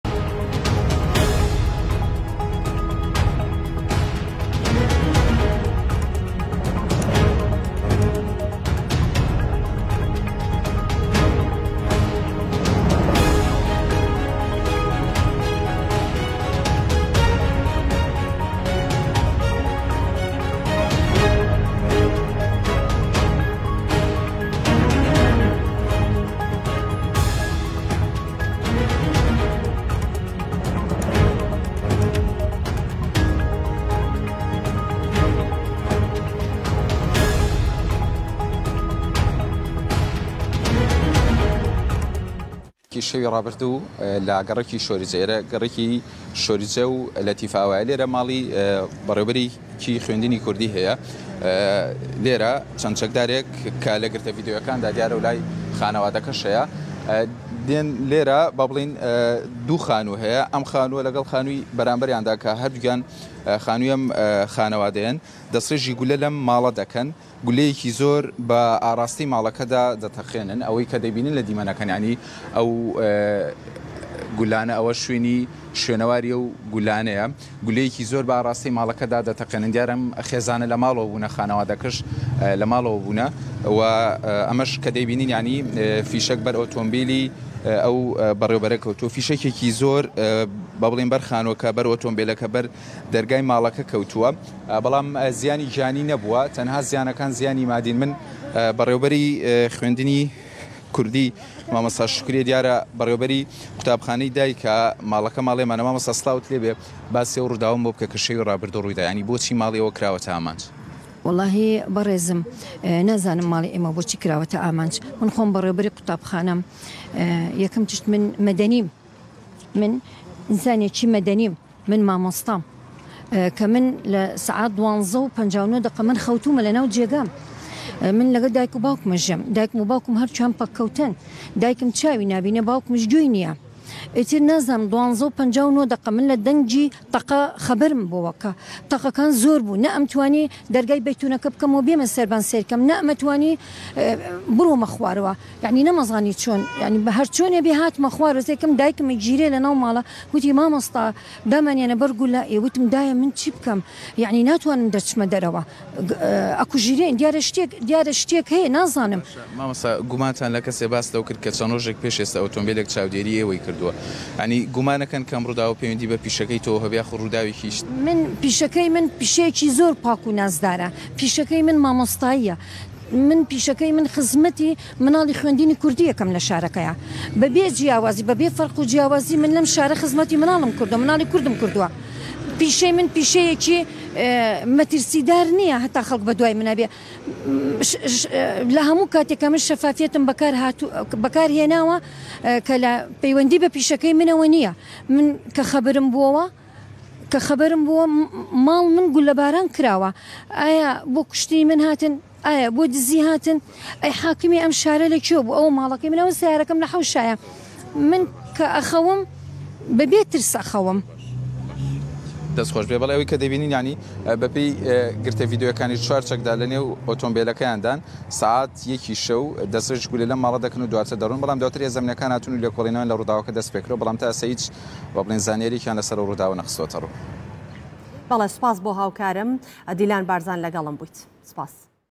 Kirkuk: Unidentified gunmen shoot a Kurdish school principal
Kirkuk, January 21st 2023 - The attacks against the Kurdish citizens in the city of Kirkuk have increased recently, and these attacks targeted Kurdish farmers and citizens, as well as the Kurdish police and lawyers. This time, unidentified gunmen opened fire on the house of the principal of a school for Kurdish studies in the Shorja district.  Attacks against the Kurds of Kirkuk have become continuous since the events of the sixteenth of October 2017, after the Iraqi forces took control of the city,  since that time, the Kurdish citizens began to leave the city for security reasons and because of racial discrimination against them.